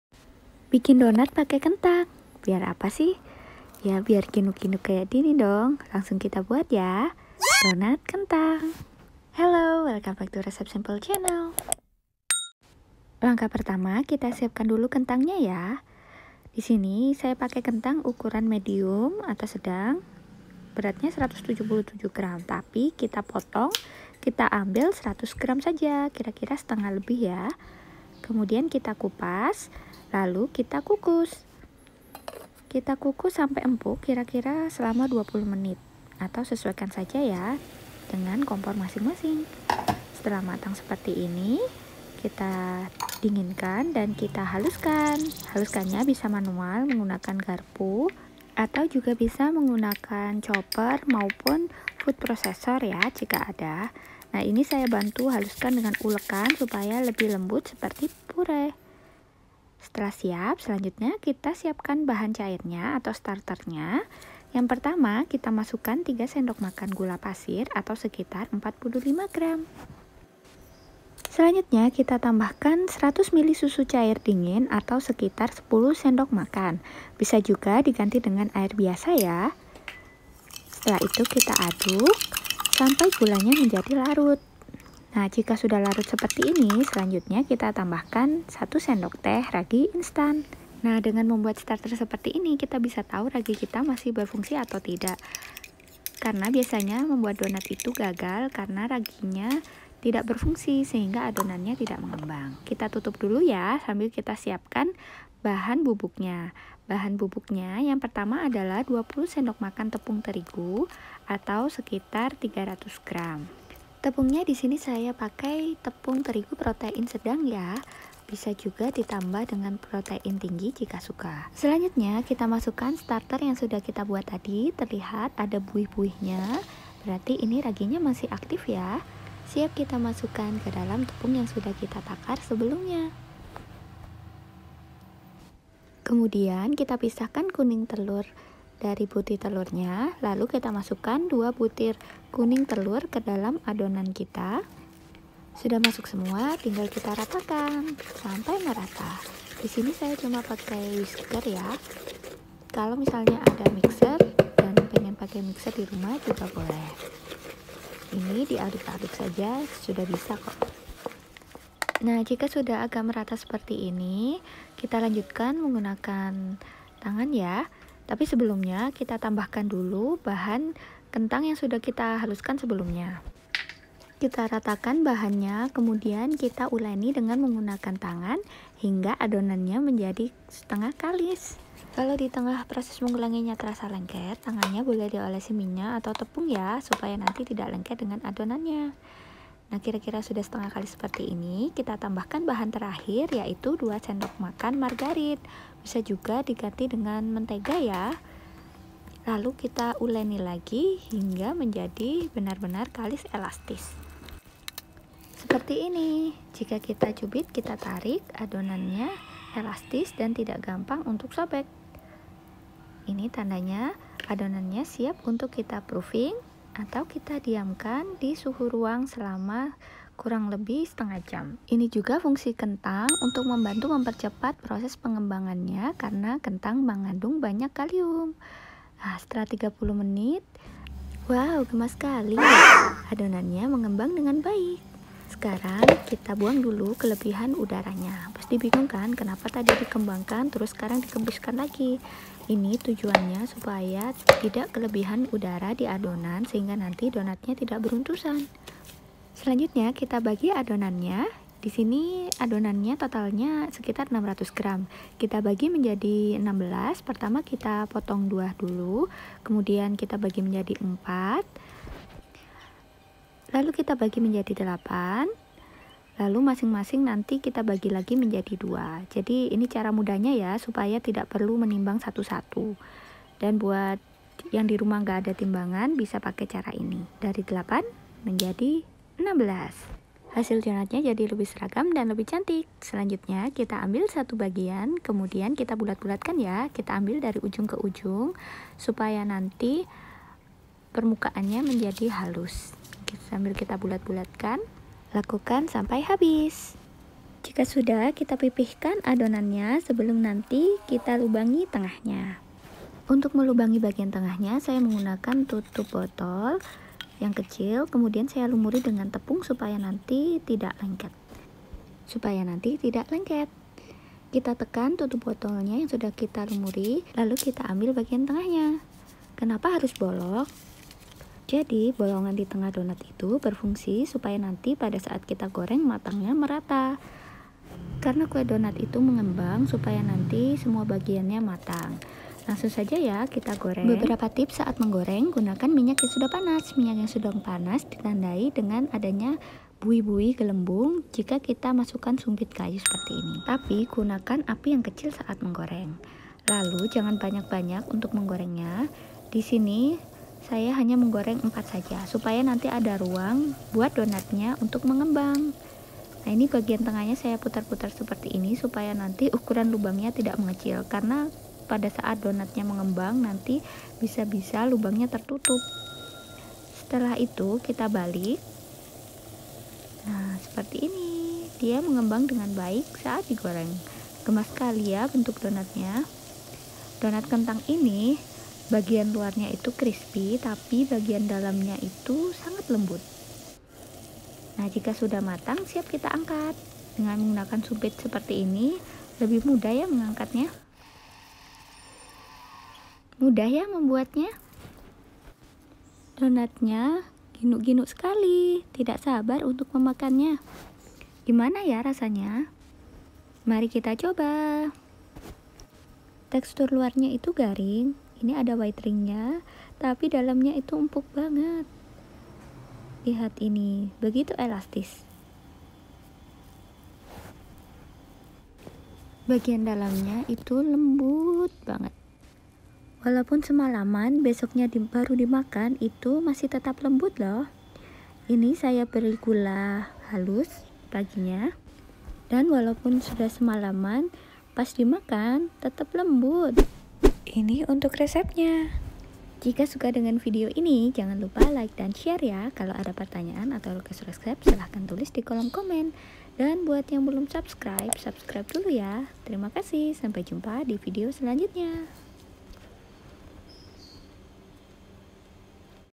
Cara Mudah Membuat Donat Kentang dengan Takaran Sendok No Mixer
Di video ini disajikan cara membuat donat kentang yang muda dengan menggunakan takaran sendok dan tanpa menggunakan mixer.

DONAT KENTANG
100 g kentang (1/2 buah kentang ukuran medium)
Bahan Cair (Starter):
100 ml susu (10 sdm)
3 sdm gula (45 g)
1 sdt ragi instant (5 g)
Bahan Bubuk
20 sdm Tepung Terigu (protein sedang) (300 g)
Bahan lemak
2 kuning telur
2 sdm margarine